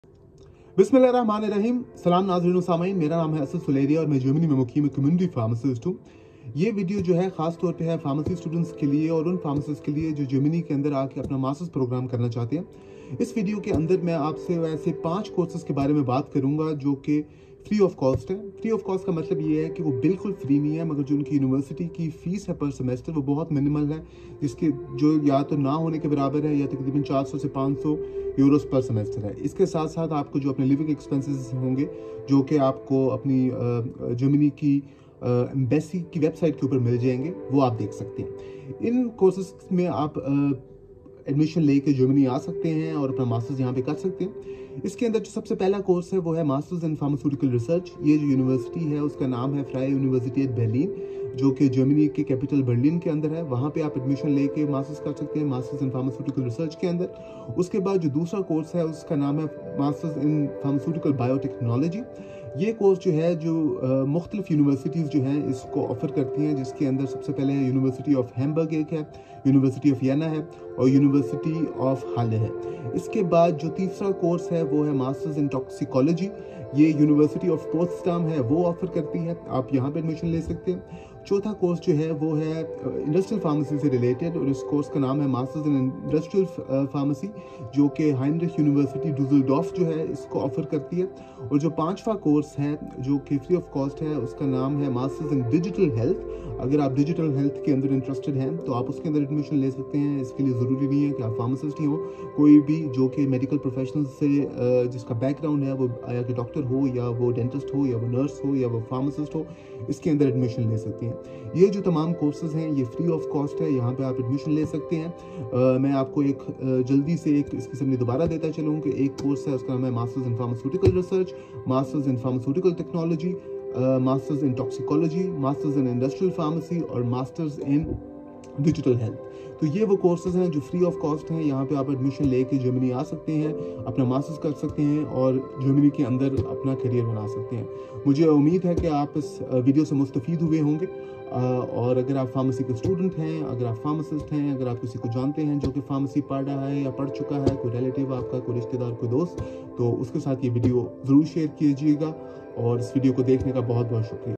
Free Master Programmes for Pharmacists in Germany
In this video I have discussed 5 courses for pharmacist which are free of cost

Masters in Pharmaceutical Research

Masters in Pharmaceutical Biotechnology

Masters In Digital Health

Masters In Industrial Pharmacy